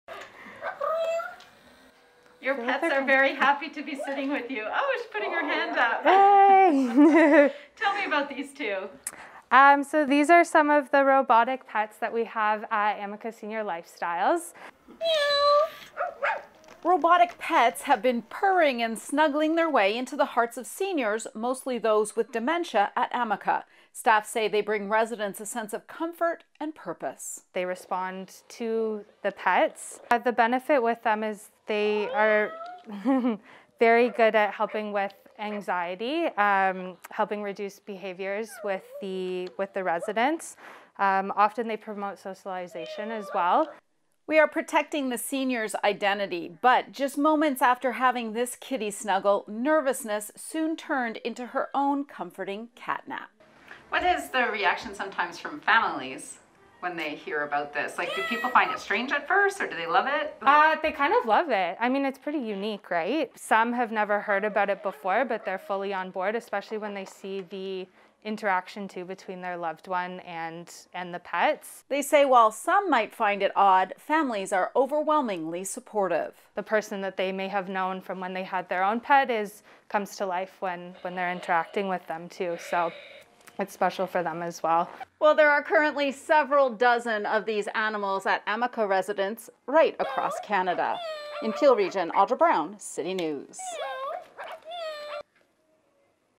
Robotic pets bringing comfort to seniors
A unique program at Amica residences across Canada provides furry robotic pets to seniors.  As Audra Brown explains, the animatronic animals provide comfort and companionship to those living with dementia.